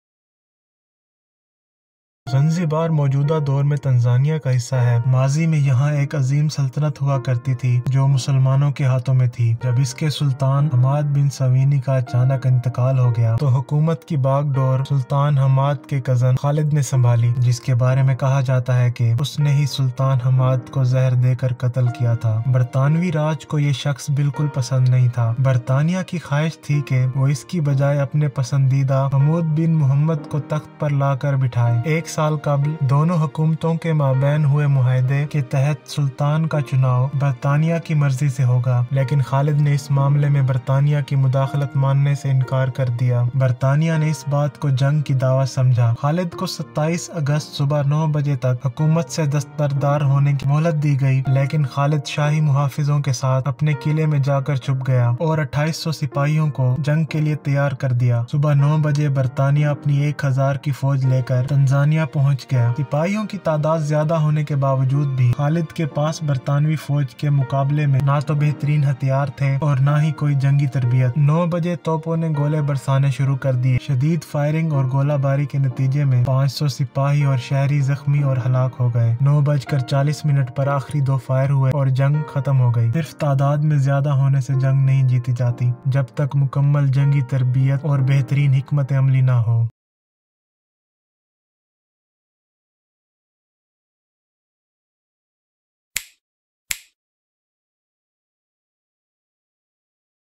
The shortest battle in history of 40 minutes
The war between Britain and the Zanzibar Empire lasted for a very short time, but 500 people, including civilians, were killed and wounded.

 Zanzibar is now part of Tanzania, but at the time it had a great empire in Muslim hands.

 Two days before the war, Sultan Hammad bin Thawini of Zanzibar died suddenly.  The government was taken over by Khalid bin Bargash, the cousin of Sultan (who is said to have poisoned the Sultan).  But the British Raj did not like this person.  The British wanted him to replace their favorite Hamud bin Muhammad with the throne.
A year ago, the two governments reached an agreement under which the final decision on the election of Zanzibar Sultan was in the hands of Britain.  But Khalid refused to accept British intervention in the matter.  Britain invited him to the war.

 Khalid was given a respite from the government at 9 am on August 27, but Khalid escaped with his bodyguards to his fort.  And 2,800 soldiers prepared for battle.  At 9 o'clock in the morning, Britain arrived with an army of 1000 men.  Despite the large number of soldiers, Zanzibar had neither the best weapons nor combat training compared to the British army.

 At exactly 9 o'clock the cannons were ordered to fire and the cannons started firing.  500 Zanzibar soldiers and civilians were injured and killed in indiscriminate firing and unprovoked attacks.  At 9:40 a.m. the last two fires were fired and the battle ended.